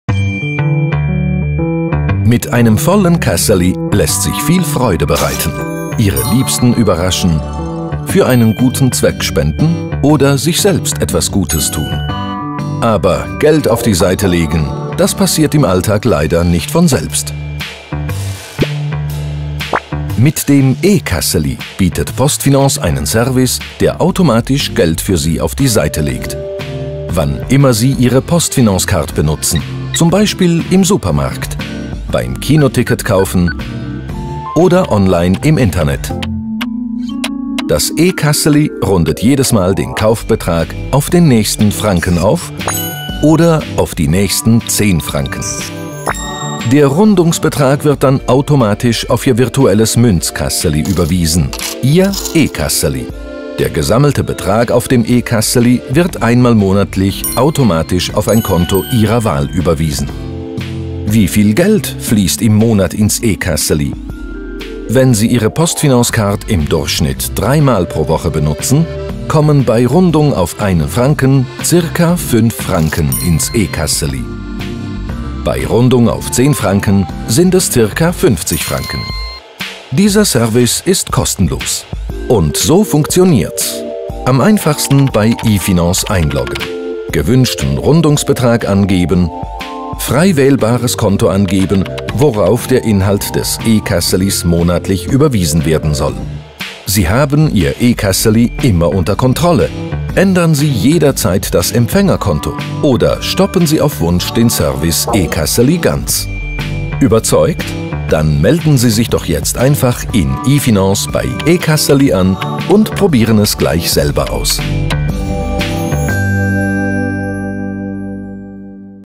PostFinance E-Kässeli
Animation film we've done for PostFinance to explain their new product E-Kässeli.